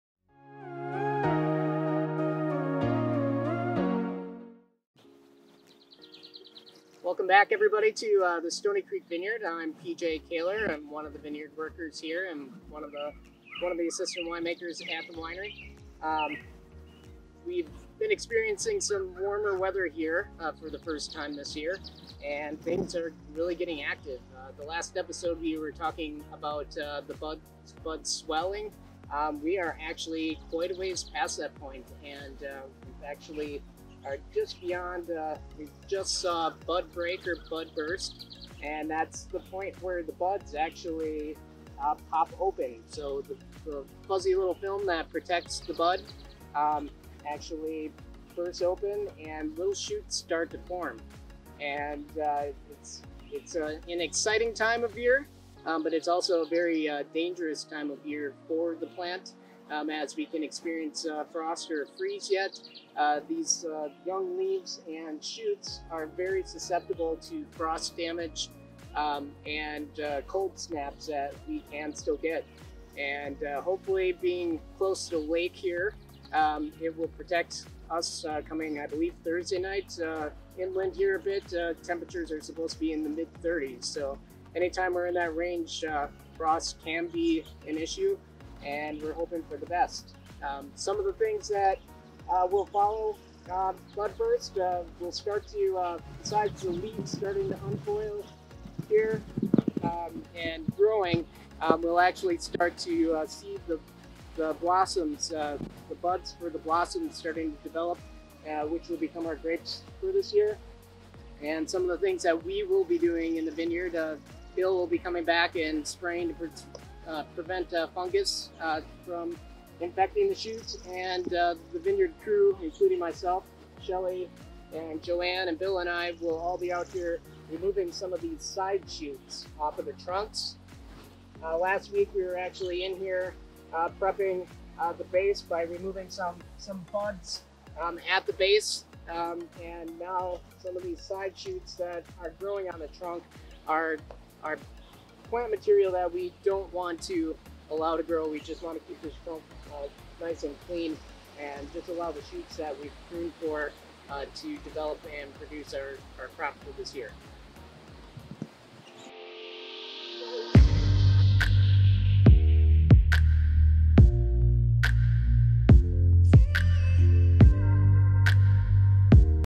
May Vineyard Update
The vineyard is starting to wake up after the warm weather we have been seeing.  PJ explains exactly what is going on in the vineyard with bud break and what we will see happen next.

or stop into the winery to learn more about the vineyard, our winery, and the delicious wines we make!